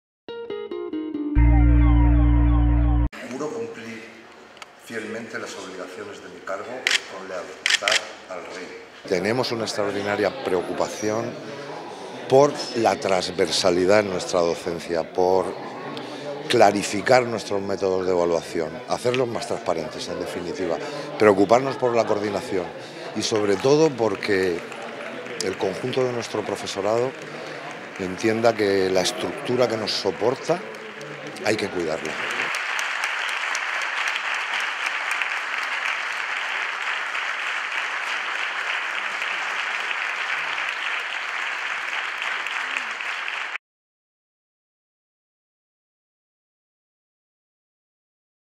José Galindo toma posesión como decano de la FBBAA - Noticia @UPVTV, 15-12-2017
José Galindo, decano de la Facultad de Bellas Artes de la UPV.

Estudió en la Facultad de la que ahora es el nuevo decano.

Doctor en Bellas Artes , profesor del Departamento de Pintura de la Facultad de Bellas Artes de la UPV desde 1989 y jefe de estudios durante 8 años con José Luis Cueto  ha realizado cuatro exposiciones individuales y varias colectivas desde 1884.

Pertenece al Centro de Formación Arte y Entorno desde su creación en 2004.